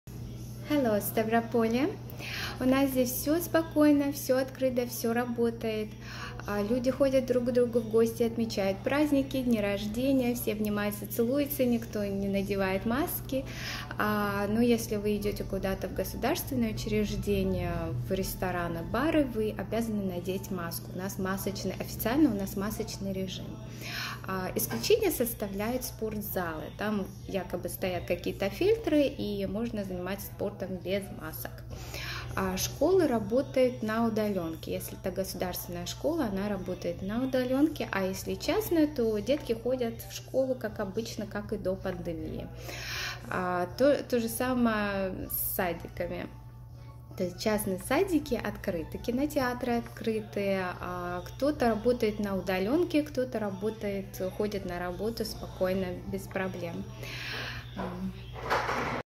HELLOСТАВРОПОЛЬЕ - Заира Фергюсон, Северная Каролина США
“HELLOСТАВРОПОЛЬЕ”! Мы снова собираем по всему миру видео от наших соотечественников и друзей с рассказами о том, что сейчас происходит в их странах в связи с пандемией коронавируса. Честные слова от хороших людей.
Сезон 2, эпизод 11 - Заира Фергюсон, Северная Каролина США.